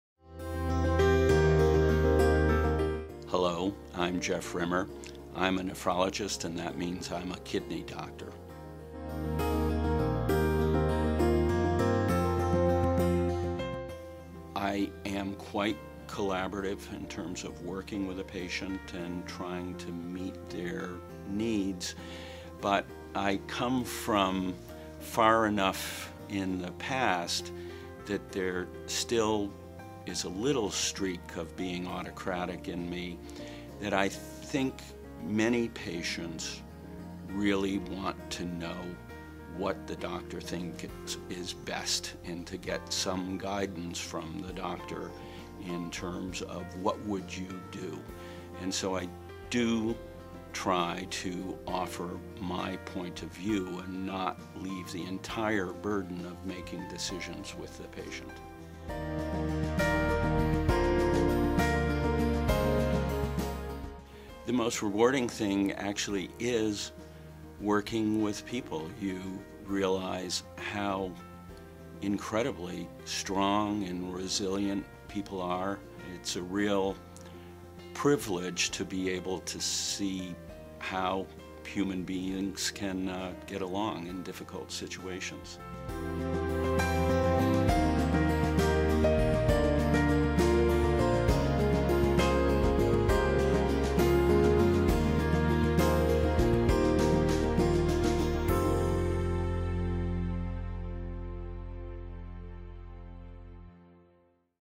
Jeffery M. Rimmer, MD, Nephrologist - Burlington VT, The UVM Medical Center
Dr. Rimmer is a nephrologist at the University of Vermont Medical Center and a professor at the Larner College of Medicine at UVM in Burlington, VT.